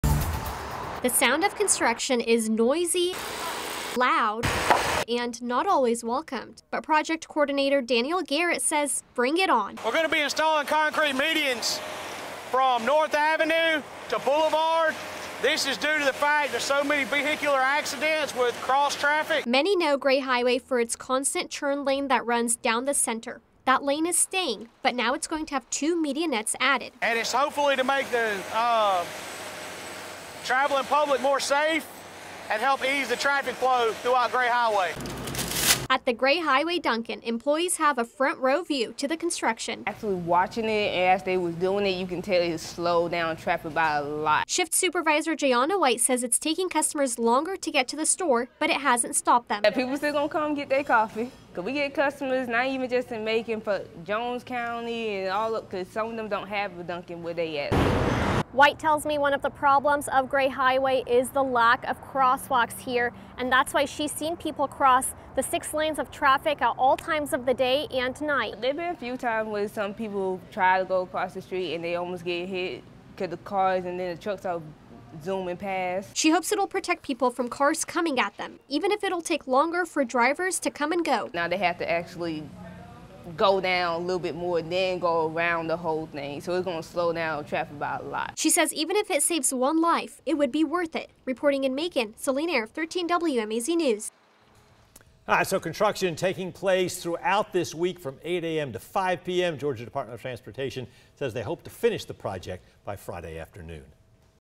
Bibb County officials adding medianettes to Gray Highway
Bibb officials said they hope the construction is complete by Friday.